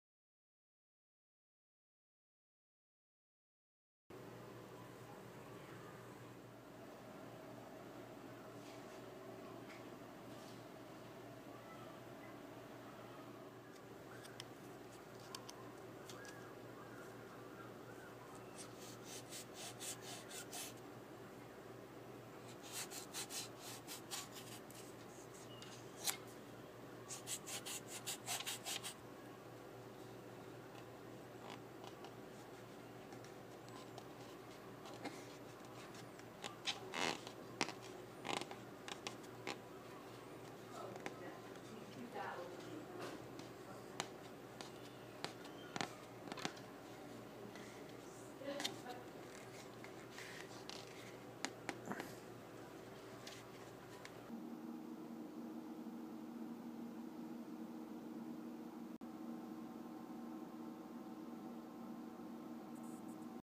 Fix a Flat Bike Tire
Here's a quick look at how to prevent a flat on road, away from home.  It's no substitute for replacing the tire, but it will get you home.